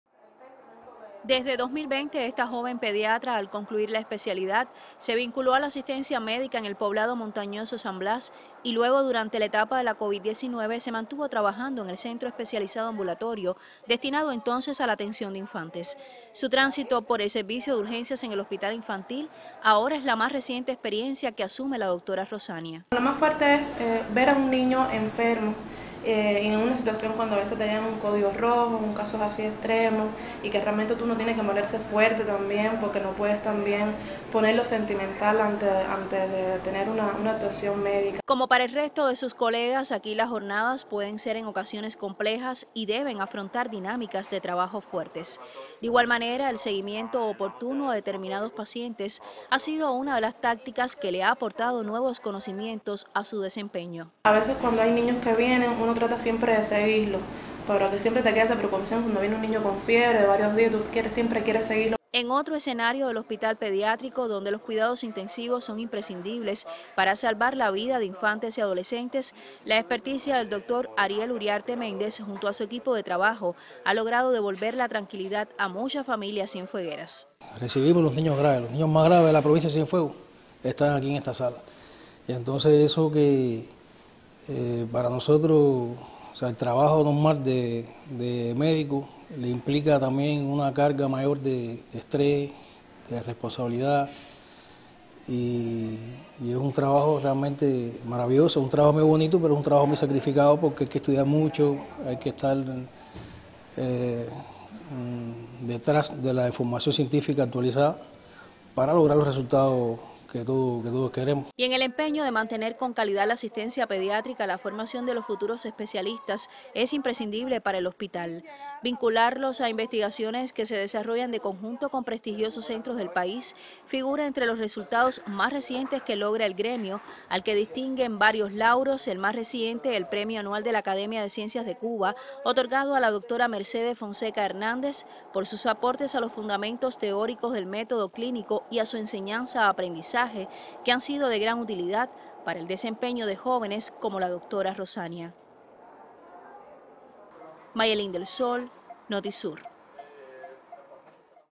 Legado en Cienfuegos en la especialidad de Pediatría
El 30 de septiembre se celebró el Día del Pediatra cubano, en homenaje al natalicio del profesor Ángel Arturo Aballi Arellano, considerado padre de la especialidad en Cuba. Conozca acerca de su legado en la dinámica que viven los profesionales en el Hospital Paquito González Cueto de Cienfuegos.